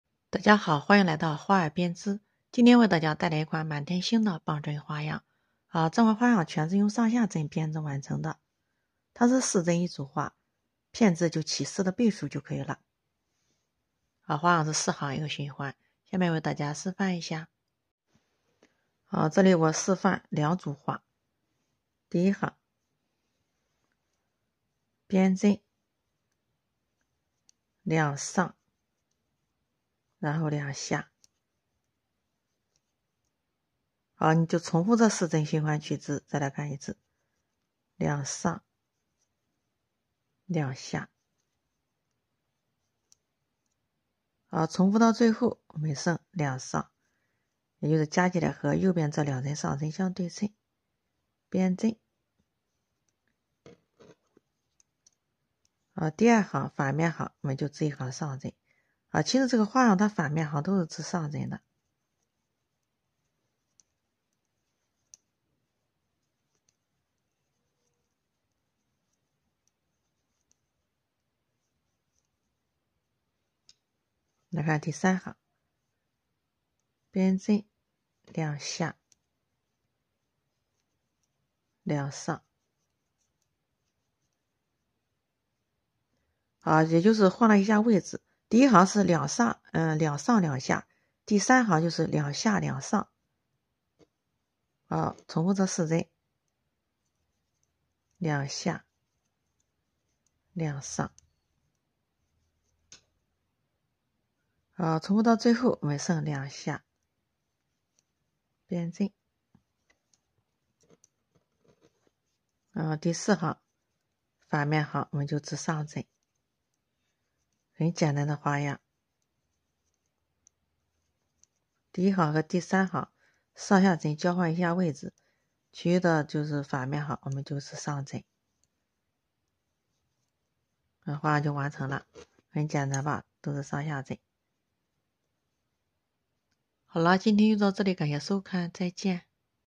簡單又易學的花樣豈能不學，小雲朵花樣教程，輕鬆就能學會
簡單又易學的花樣豈能不學，小雲朵花樣教程，輕鬆就能學會 | How can you not learn the simple and easy -to -learn tricks? Xiaoyun's trick tutorial, you can learn easily